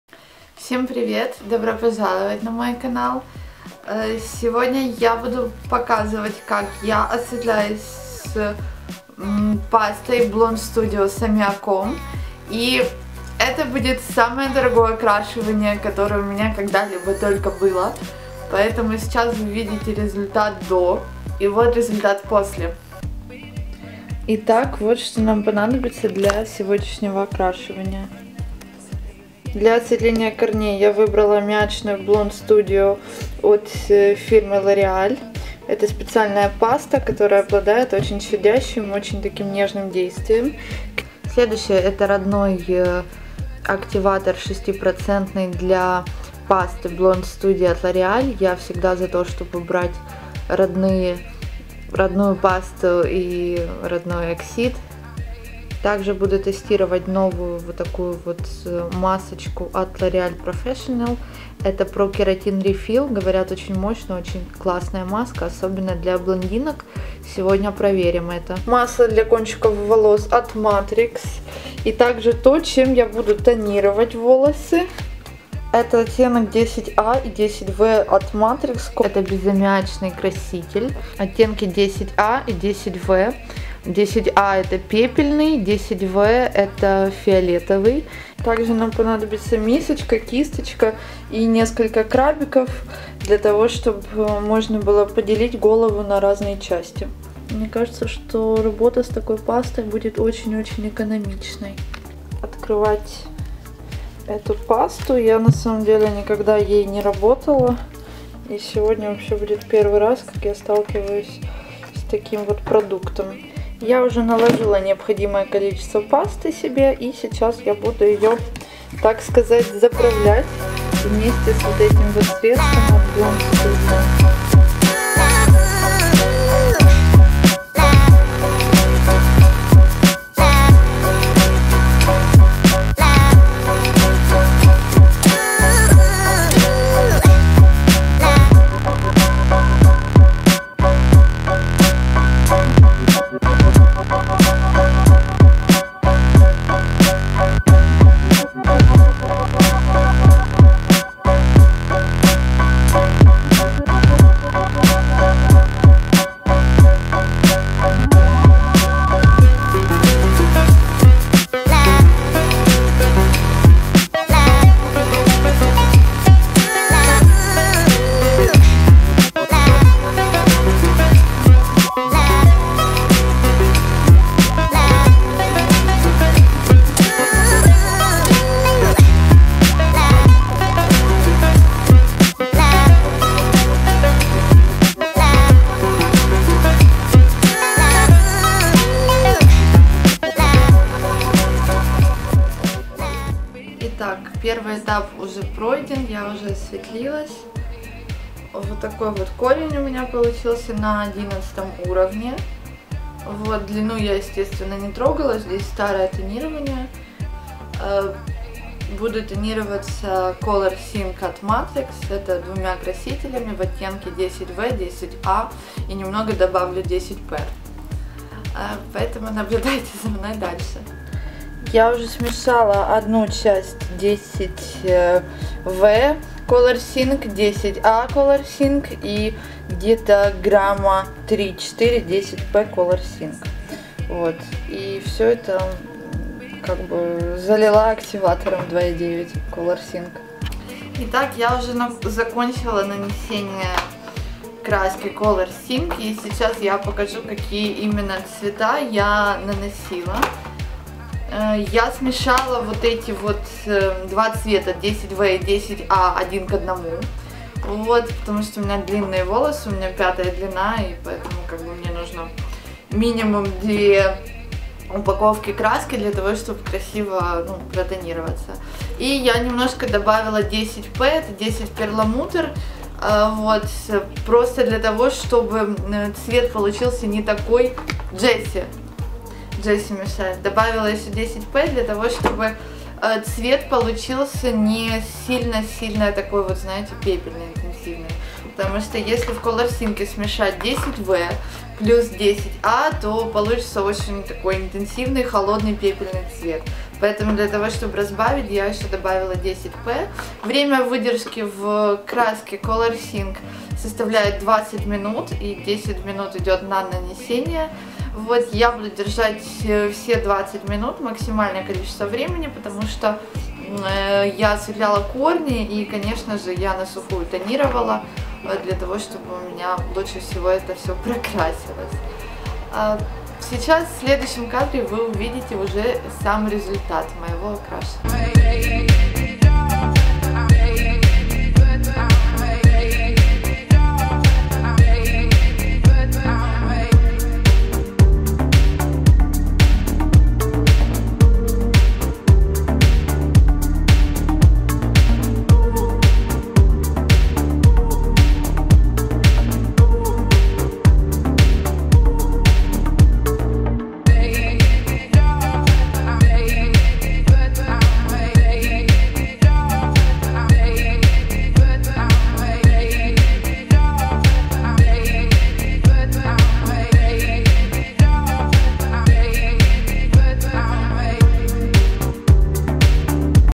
ДОРОГОЕ ОКРАШИВАНИЕ ВОЛОС 2017 // ПЕПЕЛЬНЫЙ БЛОНД В ДОМАШНИХ УСЛОВИЯХ
Окрашивание волос в холодный, красивый и натуральный оттенок в домашних условиях - теперь легко. Все блондинки мечтают добиться пепельного, холодного оттенка блонд без лишней желтизны на волосах. Не так ли?

В этом видео я покажу как сделать красивое и трендовое окрашивание волос 2017 в холодны платиновый блонд. В своей работе я буду использовать

1. Паста для осветления корней L'Oréal Professionnel Blond Studio PLUS c аммиаком
2. loreal professional nutri developer 6 %
3. Маска для волос Loreal Pro-Keratin Refill Mask
4. Масло для волос Matrix Amozonian murumuru
5. Matrix Color Sync 10 a, 10 v